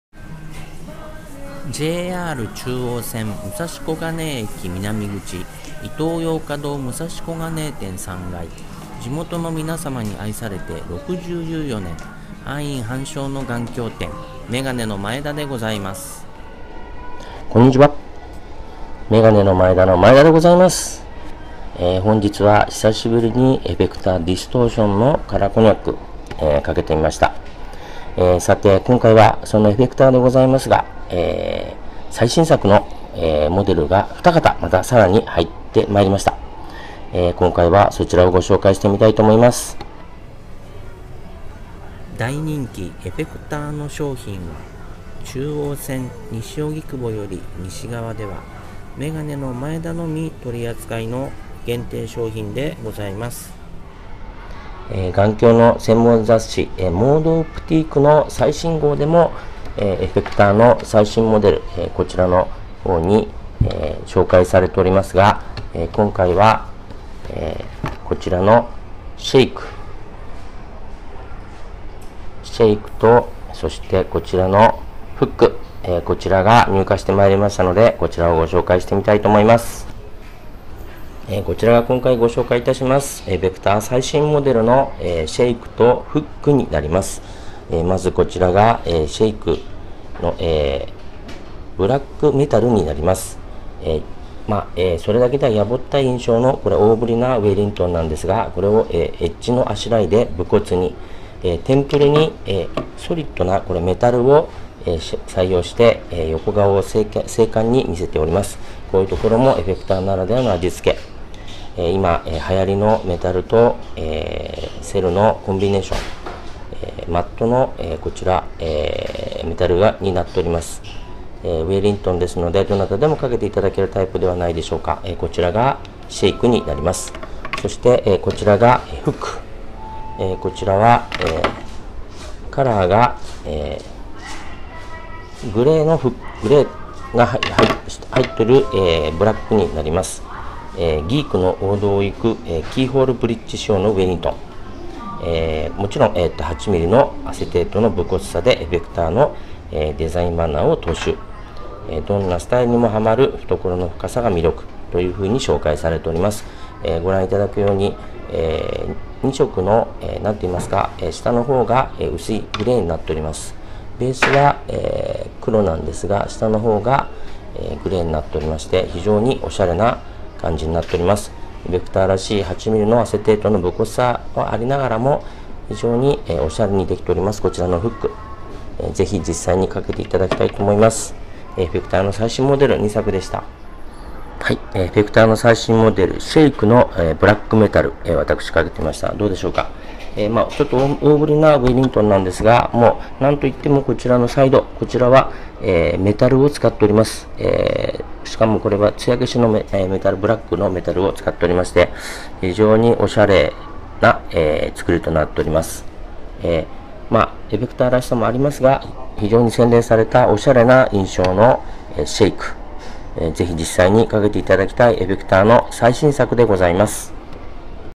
EFFECTOR の最新フレームがさらに２モデル入荷!! (小金井市 眼鏡)
東京の小金井市、JR中央線「武蔵小金井」駅南口　イトーヨーカドー３階にあるメガネのマエダです。

大勢の芸能人、スポーツ選手が愛用するEFFECTOR。

中央線西荻窪以西はメガネのマエダのみ取扱い。

今回ご紹介するのは、最新モデルの shake 、hook。

4月のEFFECTORの展示会で発表された最新６モデルのうち、本日入荷したモデル。

shake は、それだけでは野暮ったい印象の大ぶりなウェリントンをエッジのあしらいで無骨に。テンプルにソリッドなメタルを採用して、横顔を精悍に見せるのもエフェクターならではの味付け。

hook は、ギークの王道をいく、キーホールブリッジ仕様のウェリントン。もちろん８ｍｍアセテートの無骨さでエフェクターのデザインマナーを踏襲。どんなスタイルにもハマる懐の深さが魅力。

眼鏡専門雑誌のMODE OPTIQUE 最新号でも掲載され、さらにEFFECTOR のホームページでも紹介されてます。

福井県鯖江の眼鏡職人さんが一つ一つ手作り。

その質感をぜひ店頭でお確かめ下さい。